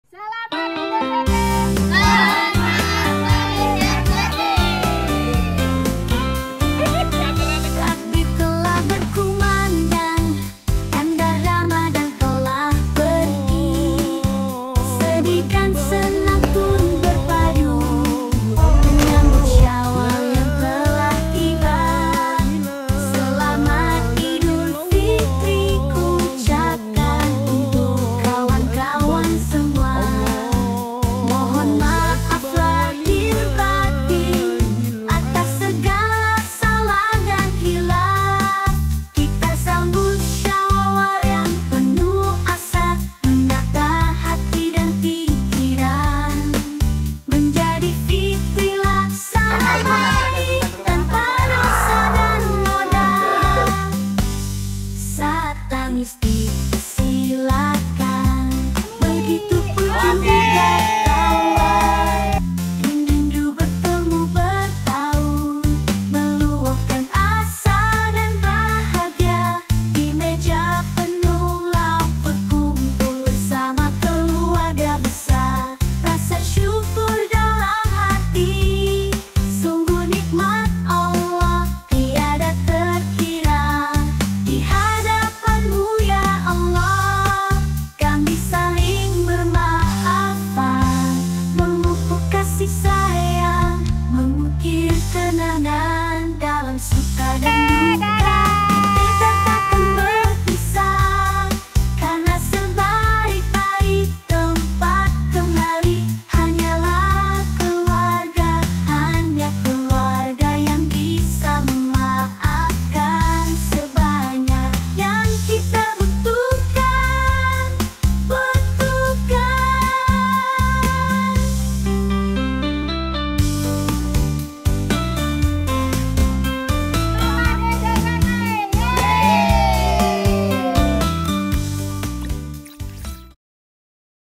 HAPPY EID MUBARAK 2024 | HAPPY LEBARAN DAY
Takbir telah berkumandang
Tanda Ramadan telah pergi
Sedih dan senang pun berpadu
Menyambut Syawal yang telah tiba
Selamat Idulfitri kuucapkan
Untuk kawan-kawan semua

Atas segala salah dan khilaf
kita sambut Syawal yang penuh asa
Menata hati dan pikiran
Menjadi fitri laksana bayi
Tanpa dosa dan noda.

Saat tangis dipersilakan
Begitupun juga tawa
Rindu bertemu bertaut
Meluapkan asa dan bahagia
Di meja penuh lauk berkumpul bersama keluarga besar
Rasa syukur dalam hati
Sungguh nikmat Allah tiada terkira

Di hadapan-Mu ya Allah
Kami saling bermaafan
Memupuk kasih sayang
Mengukir kenangan
Dalam suka dan duka
Kita takkan berpisah
Karna sebaik baik tempat kembali hanyalah keluarga
Hanya keluarga yang bisa memaafkan sebanyak yang kita butuhkan